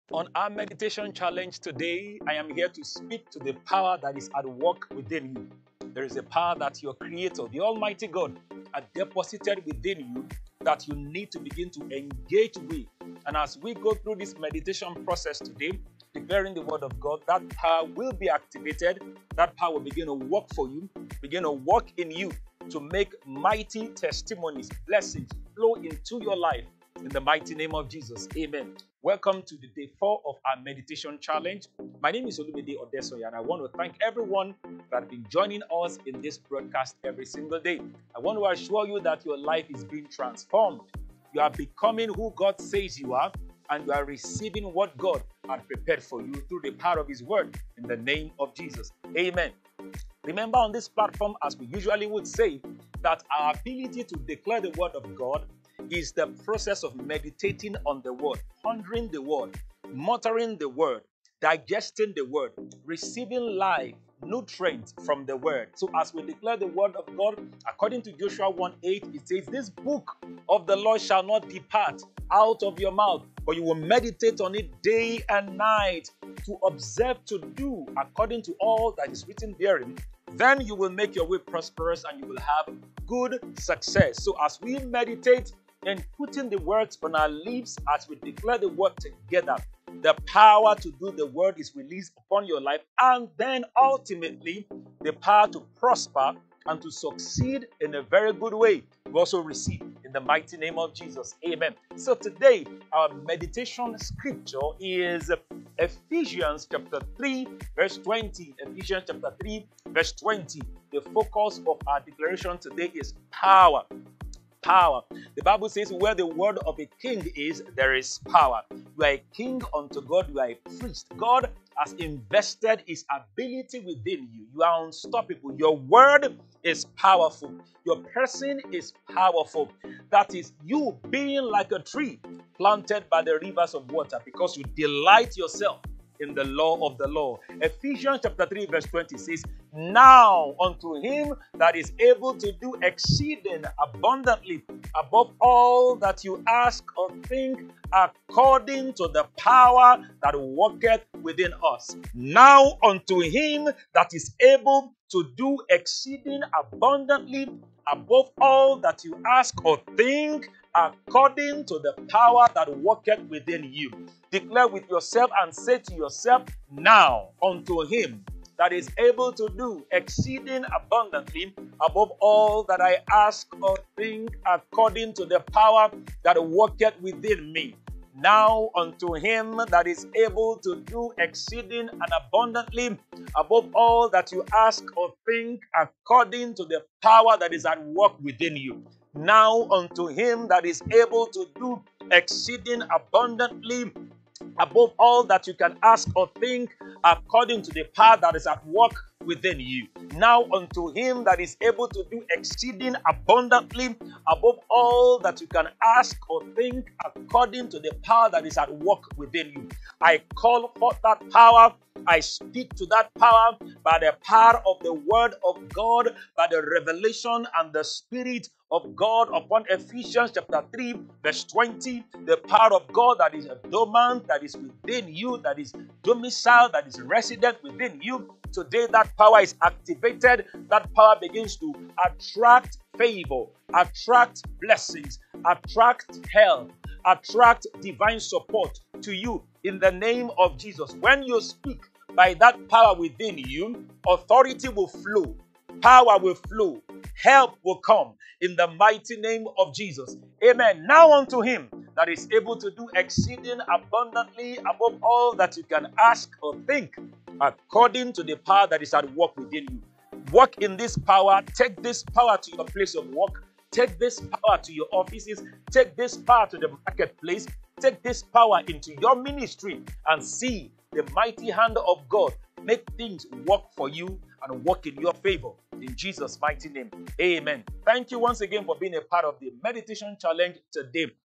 Day 4: Power // Meditation Challenge - 24/06/2023 // Olumide Odesanya
The power of meditation in God's word, transforms you into BECOMING, so as to attract abundance and blessings into your life.
Day 4: Power // Meditation Challenge with Olumide Odesanya

Join me in this 10 Day Meditation Challenge as we get ready for a more rewarding second half of the year. God bless you